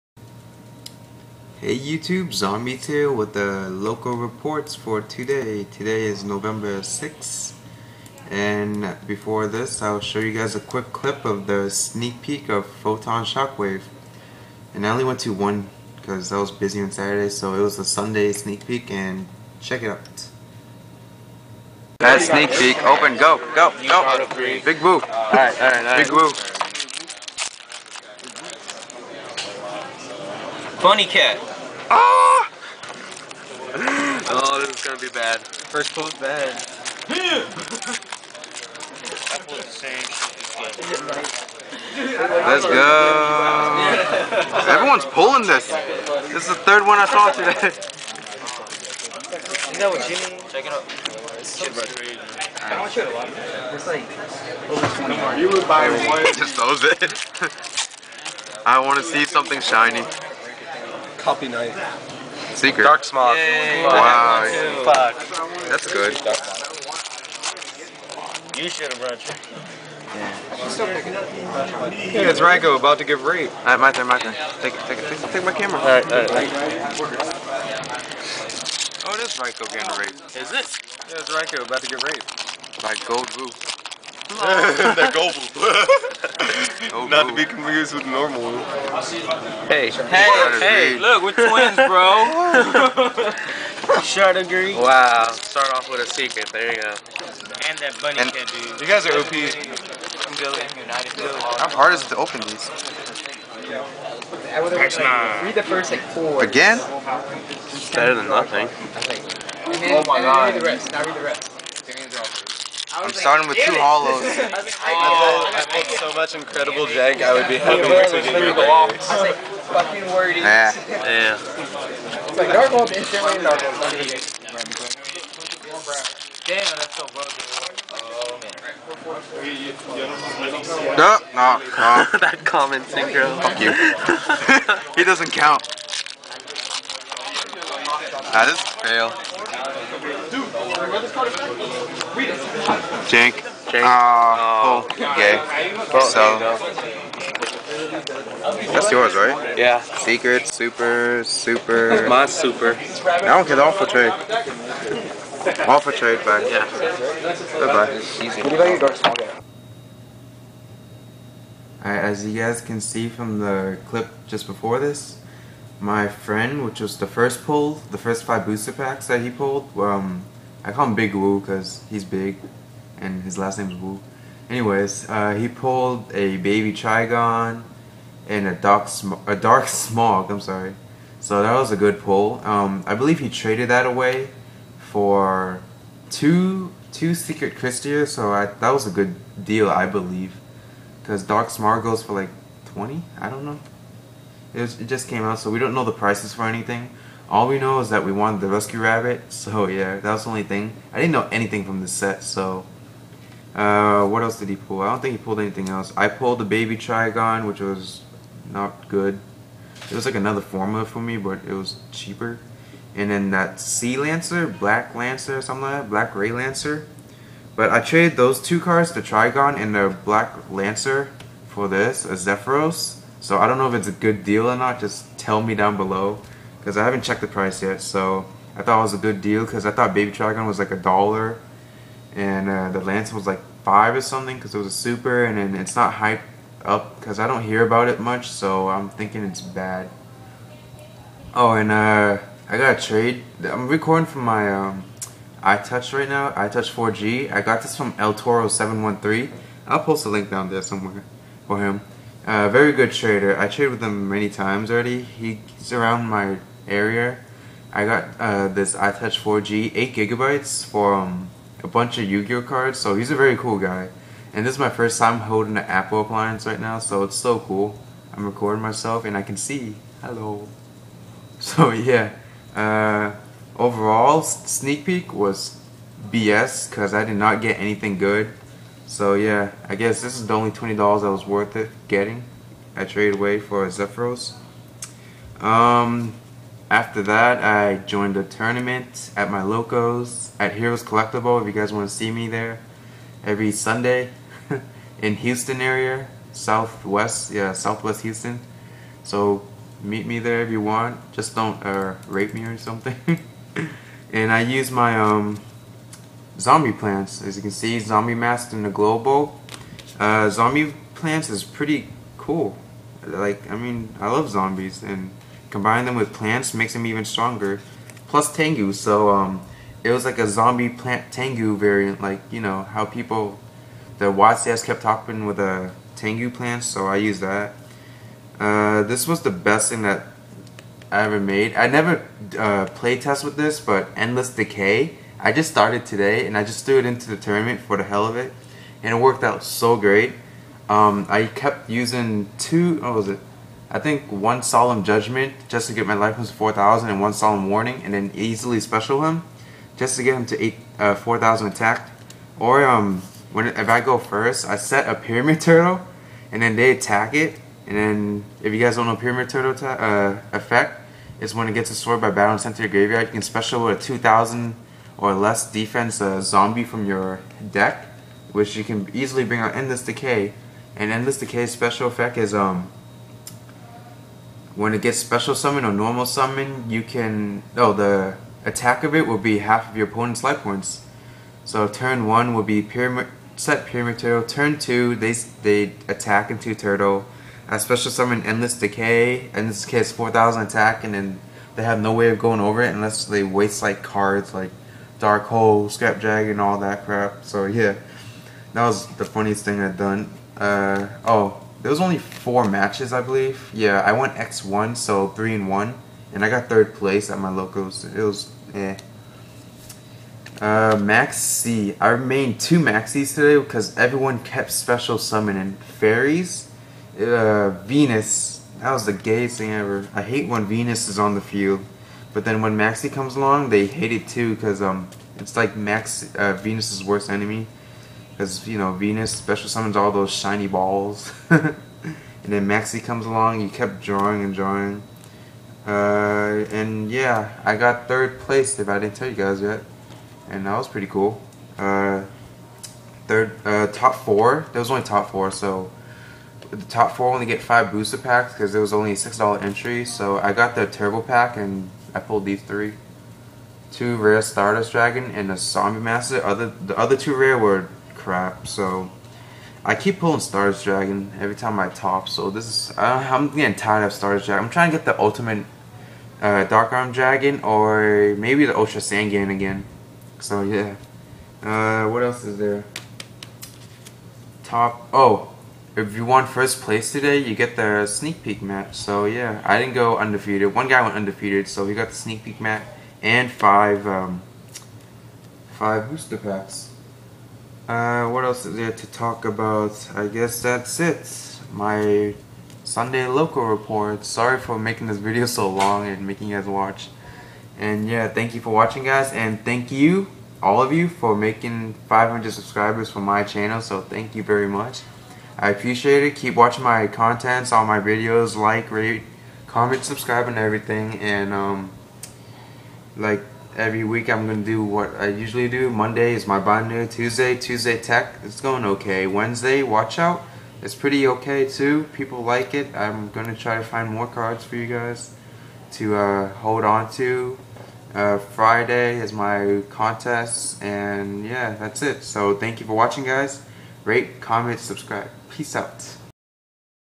VLog #2: Photon Shockwave Sneak Peek, Tourny Results!~
Sorry for making this video REALLY long. Didn't expect myself to talk this much, but there was too many things to talk about, since I won my local's tournament xD If you do watch the whole thing, I thank you~